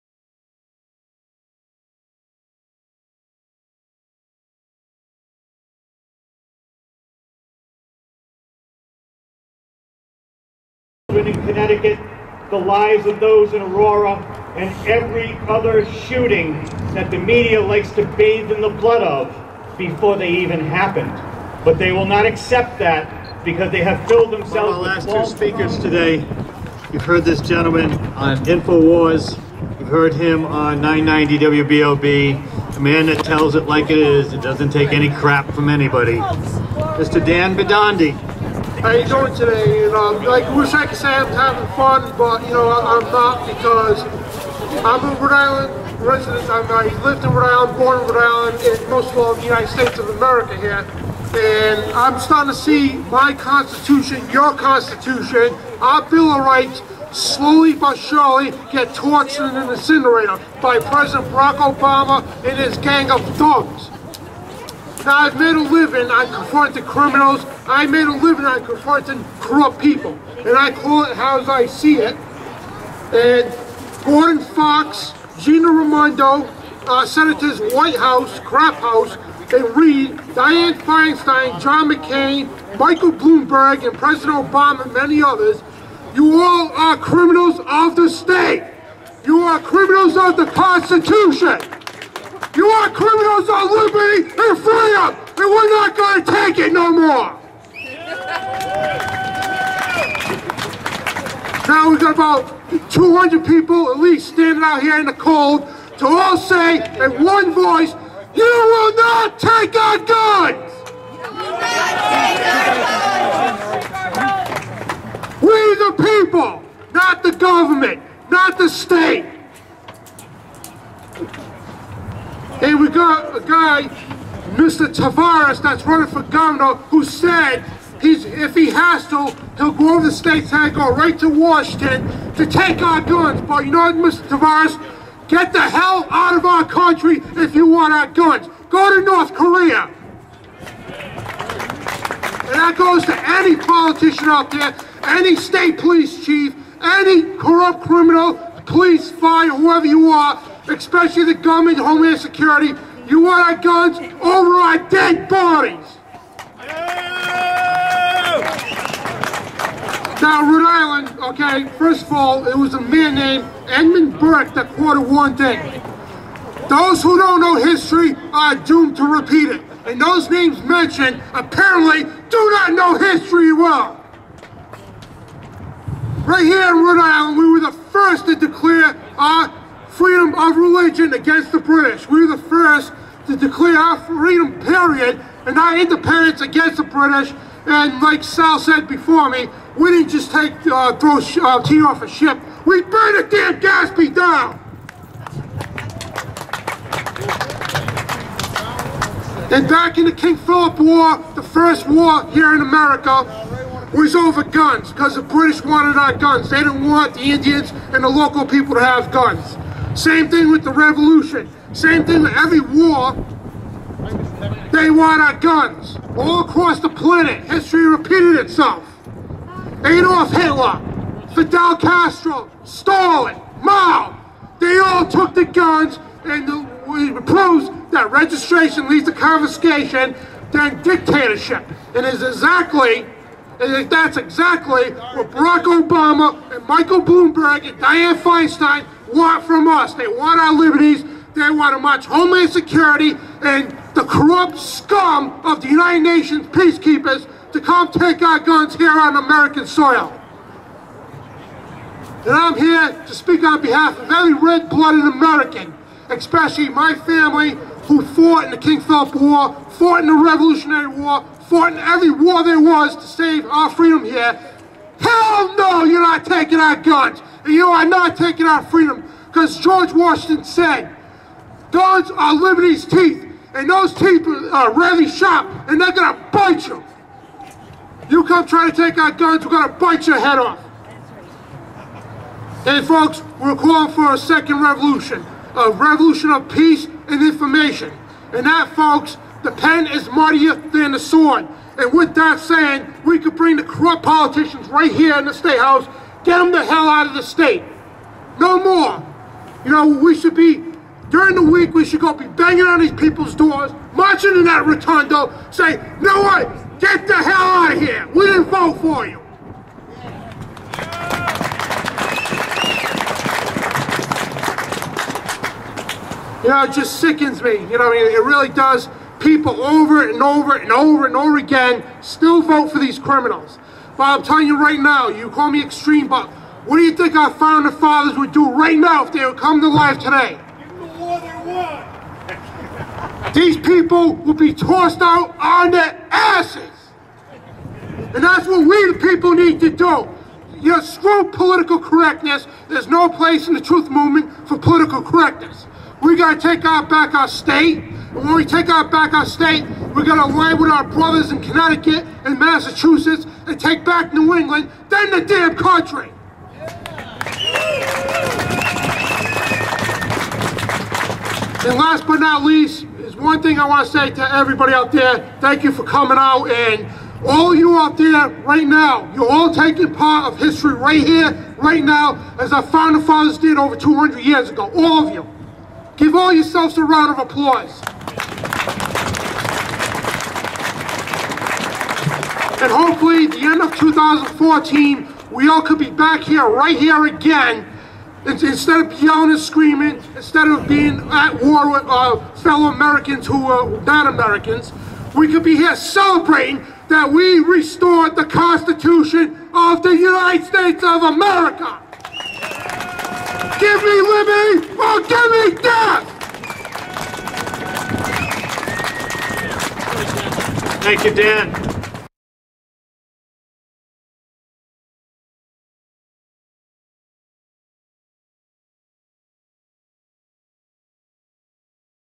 Dan Bidondi's Historic Speech At "Stand Our Ground" Pro-2nd Amendment Rally
Dan Bidondi's Historic Pro-Gun Speech At "Stand Our Ground" Pro-2nd Amendment Rally
Providence, RI
Rhode Island State House